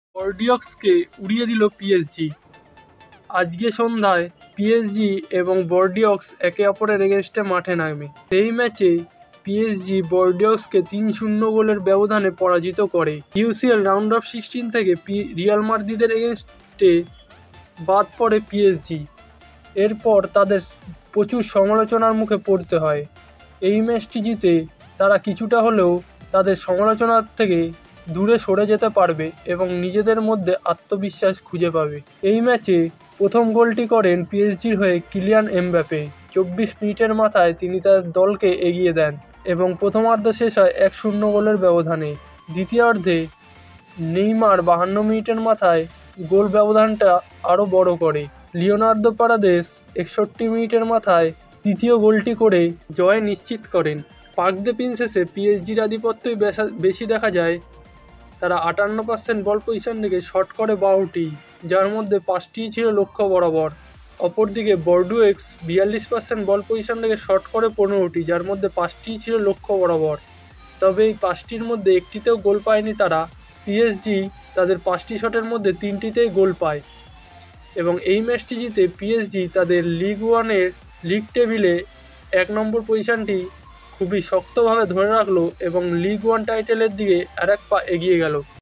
পিএসজির বড়ো জয় ❗ বর্দেয়াক্স কে গোল বন্যায় ভাসালো পিএসজি🔥🔥আবারও হতাশ মেসি🥲🥲/PSG vs Bordeaux match
This channel only for world all football league match highlights,all Hero indian super league match highlights,all world cup and international football match highlights🔥🔥